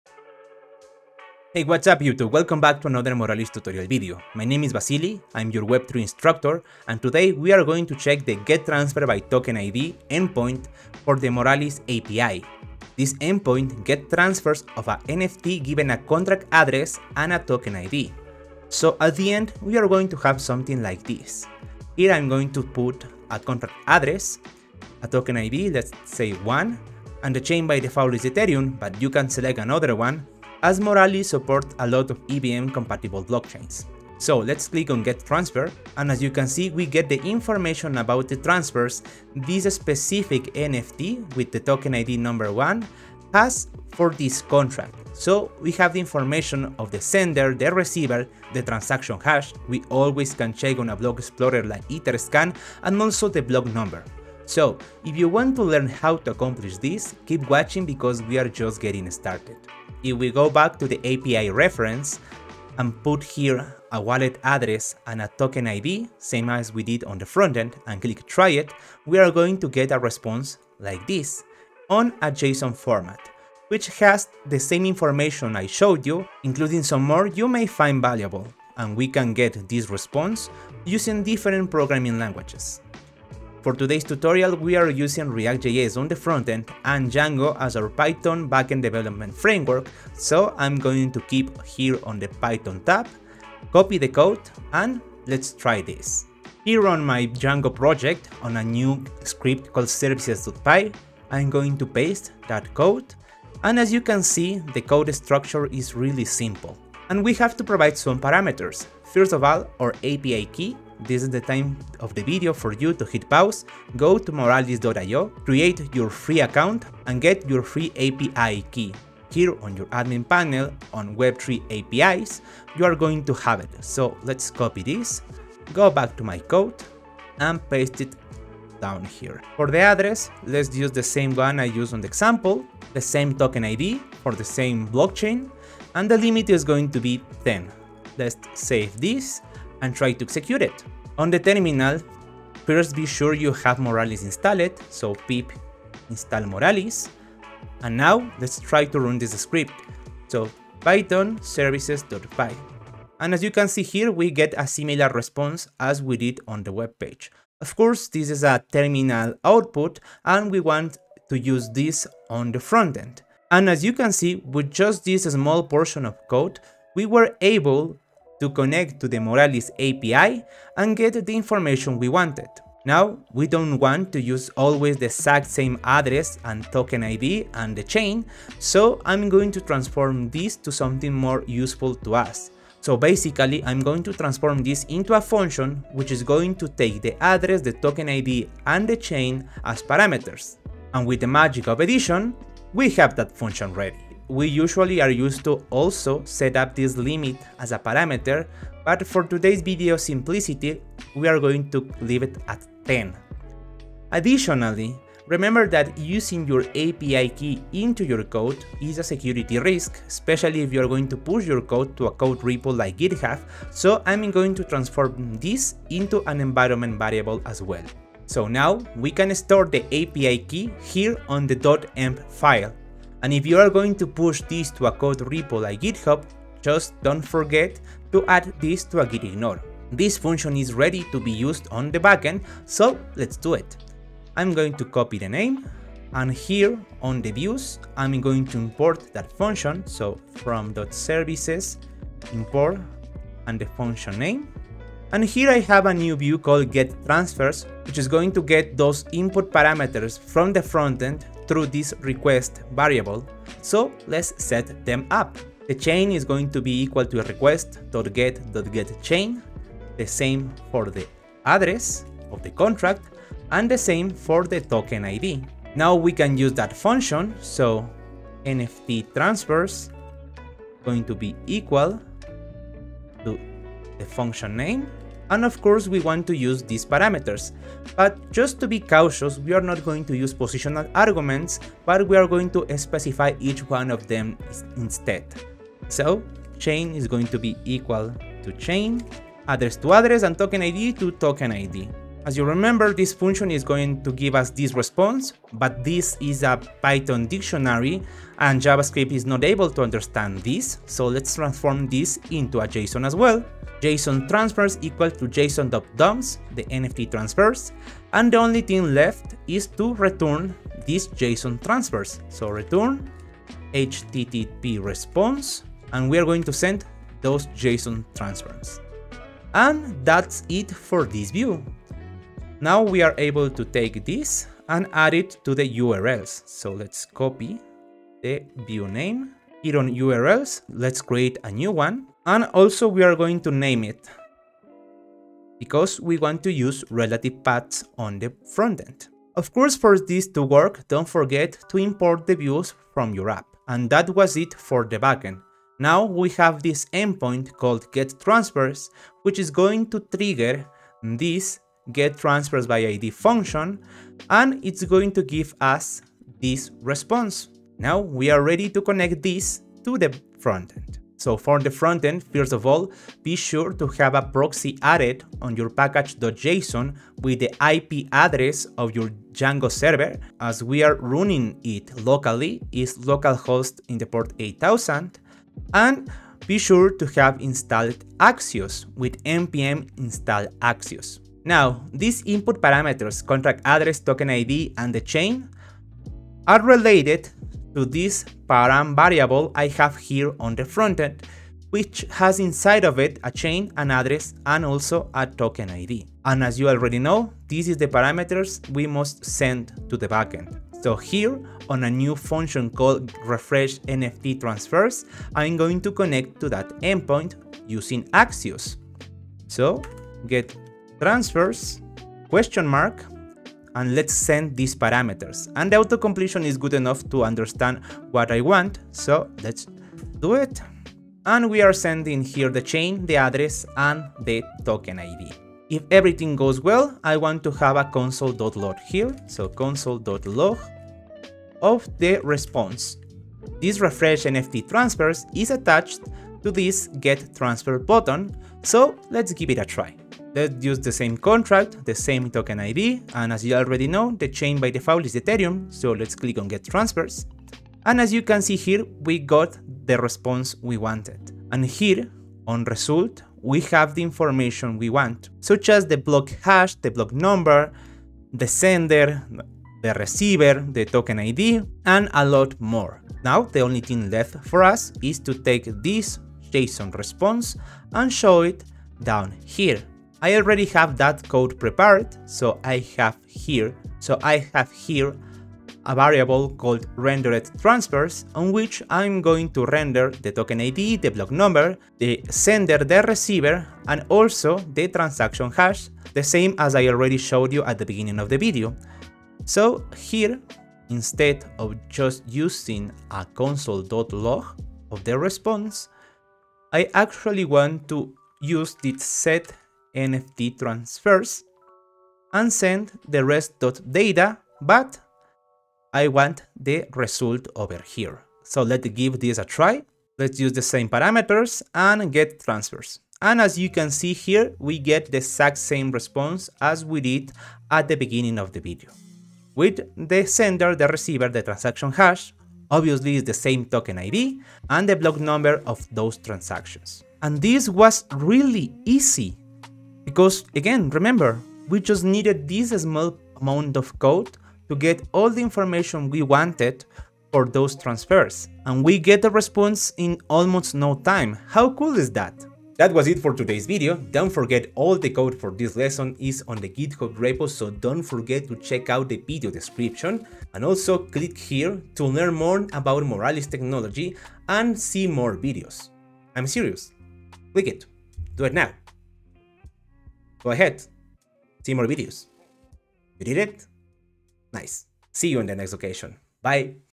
Get All NFT Transfers by Token ID| Moralis Documentation | Web3 NFT API with Python & React
Introduction to the get NFT transfers by ID |  NFT API Endpoint.